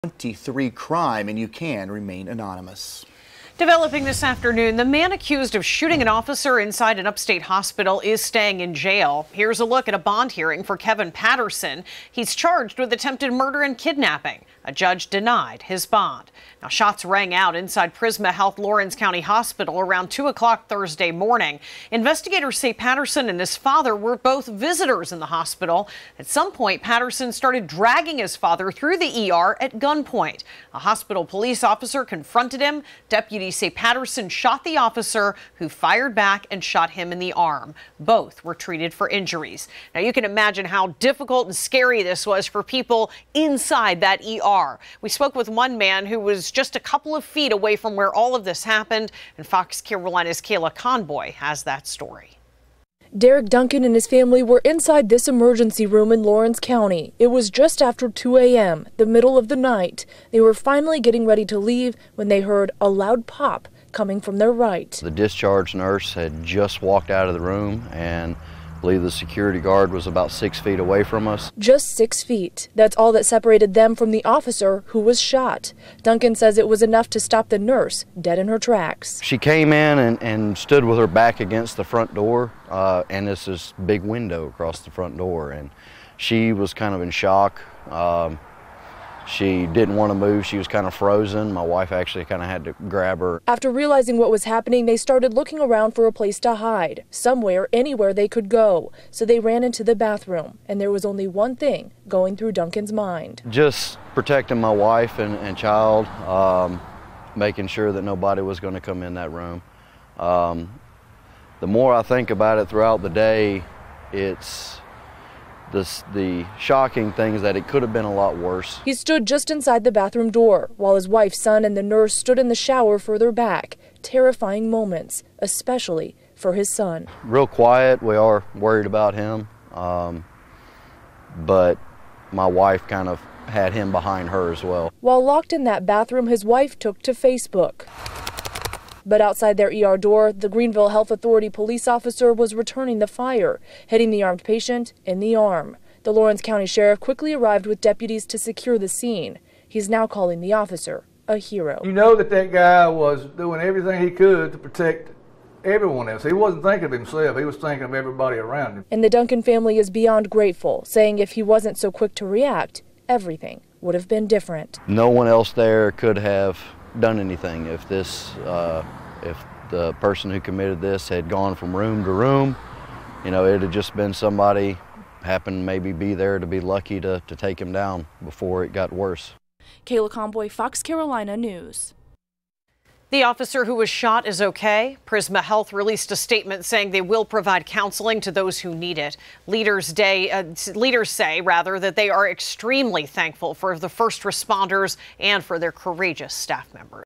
Bond denied for Laurens Co. hospital shooting suspect
FOX Carolina's Kayla Conboy has the latest.